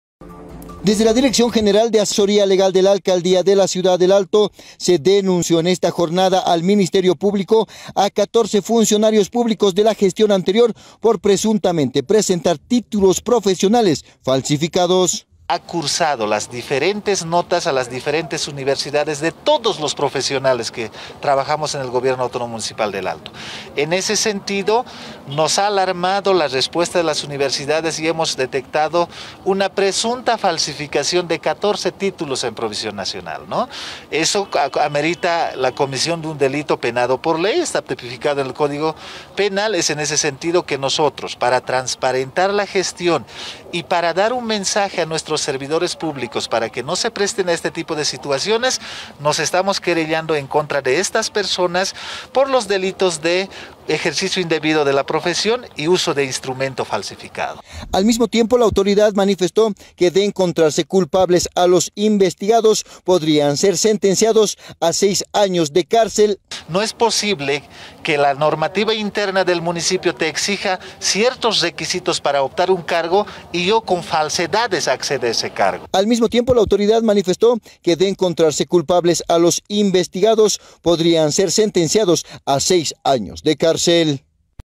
Detectan títulos profesionales falsos de 14 funcionarios de la Alcaldía. Serán procesados por dos d
ElAlto | Detectan títulos profesionales falsos de 14 funcionarios de la Alcaldía.

Serán procesados por dos delitos penados con hasta 6 años de cárcel.